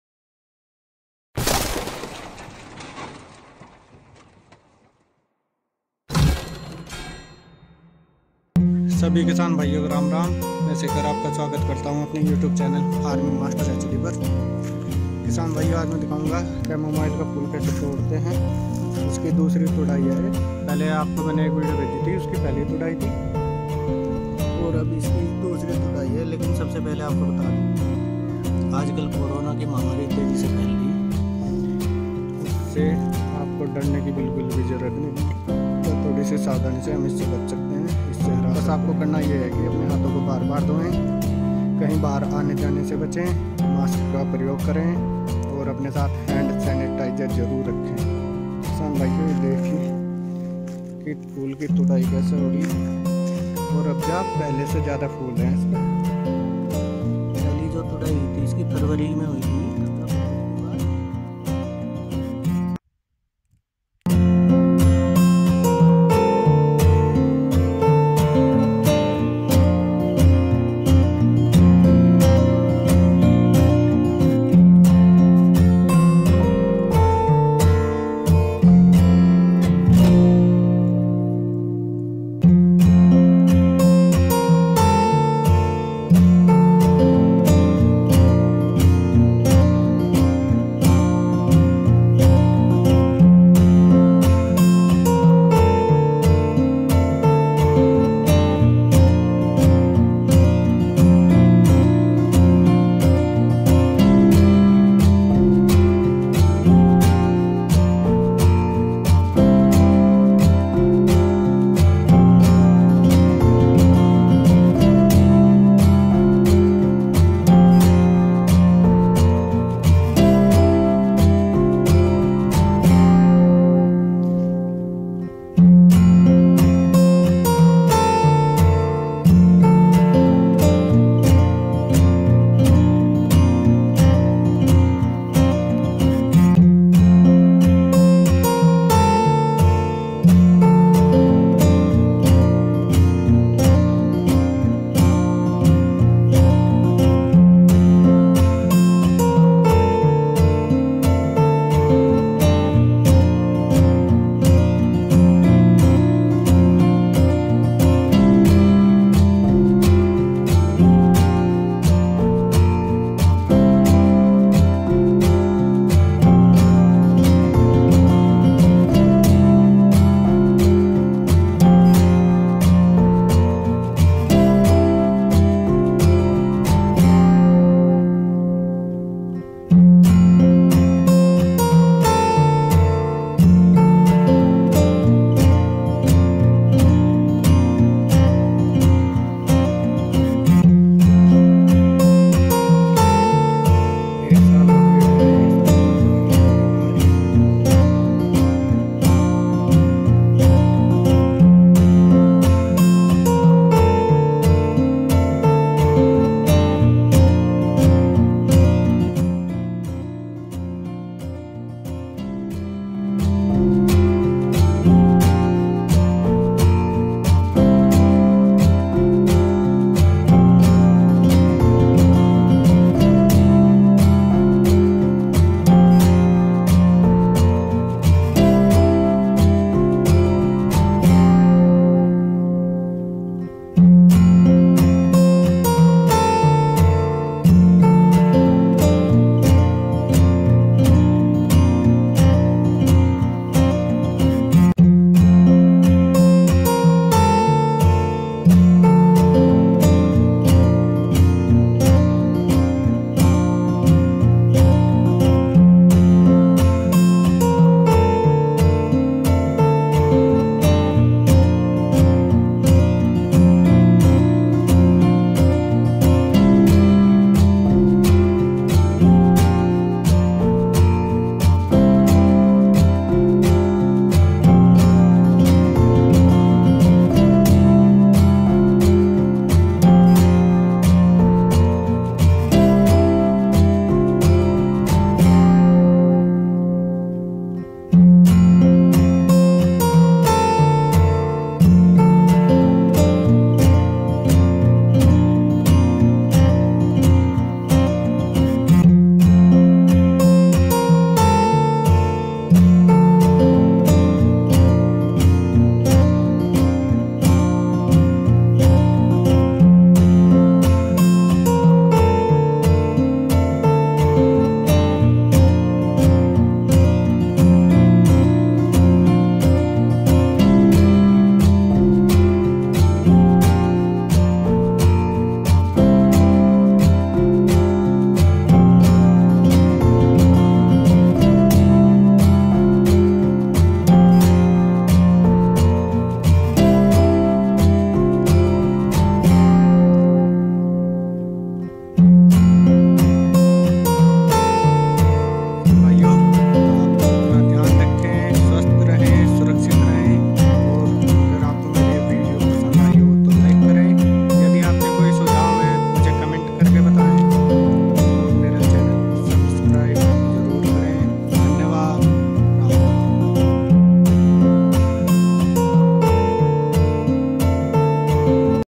Chamomile फूल की खेती कैसे करे, कैमोमाइल फूल की दूसरी तुड़ाई हुई 15 दिनों के बाद (chamomile flower)
# Chamomile , # ChamomileFlower ,
कैमोमाइल तेल को कैमोमाइल पौधे के फूलों से निकाला जाता है, कैमोमाइल दो प्रकार के होते हैं, रोमन कैमोमाइल, जिसे वैज्ञानिक रूप से एंथमिस नोबिलिस और जर्मन कैमोमाइल के रूप में जाना जाता है, जिसका वैज्ञानिक नाम मैट्रिकेरिया कैमोमिला है। दोनों किस्मों से निकाले गए तेल कुछ औषधीय गुणों में काफी समान हैं,